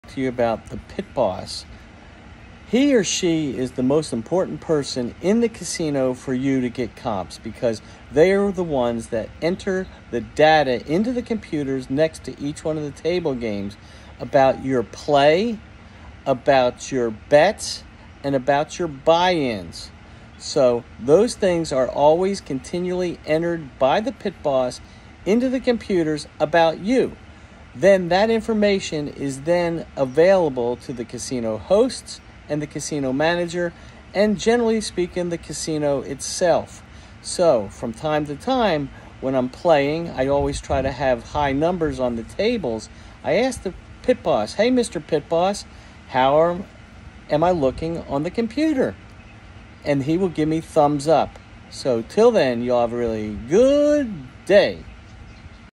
Why is the Pit Boss the most important person in the Casino to You?
The Pit Boss in the casino enters all your gambling play action into the computers next to the table games. The information they are entering about you is used by the Casino Hists and casino to give you your comps. The friendlier you are and the better tipper you are to the Pit boss dealers….the better your numbers will look on the computer. It is true. One time I was playing at Bellagio in Vegas and I played craps for like 6 hours and I put the dealers in on every players roll. This is an Mlife (MGM Property) and the Pit Biss helped to award me over 25,000 points. Normally when I would play the same way but not putting the dealers in ….I would receive like 4,000 points. It’s important to be kind to the pit boss.